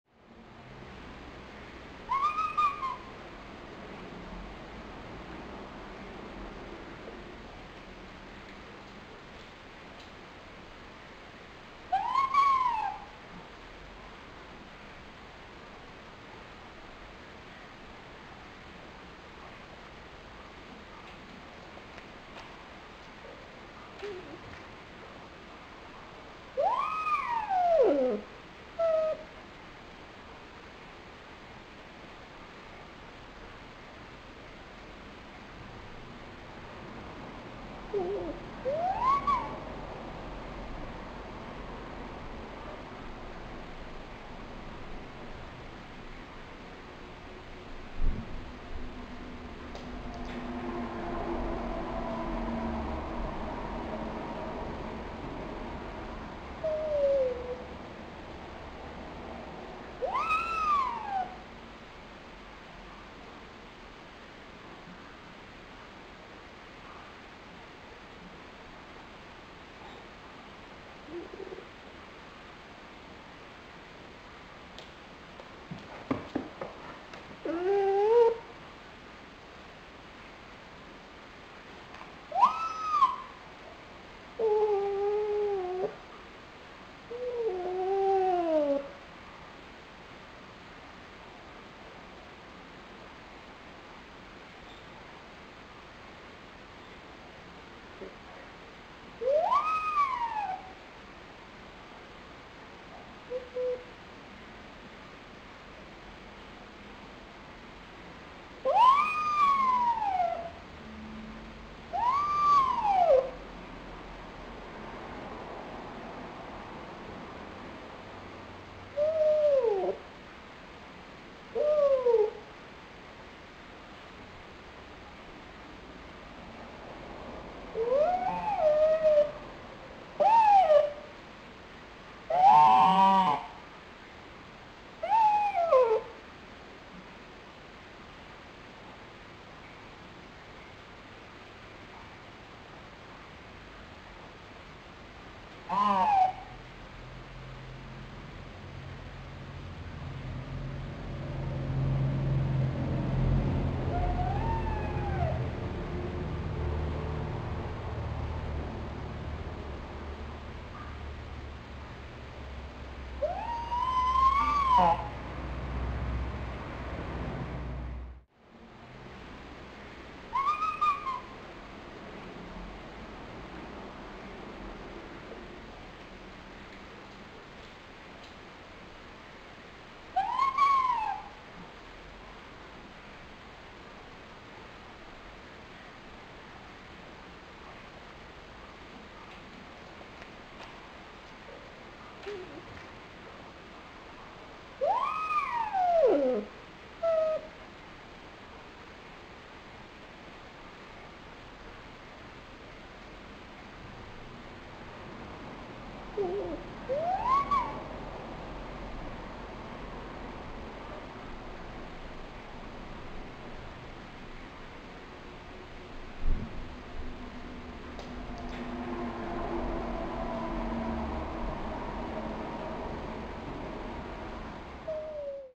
ポチの鳴き声とスライドショーPochi's call and slide show
２００７年２月１２日
ポチが生後９ヶ月の時の鳴き声です。
彼は寂しくて鳴いています。
写真も２００７年に撮ったものだけです。
Pochi is the cry at the age of nine months after birth.
He is lonely and calls.
A photograph is only the thing which I took in 2007.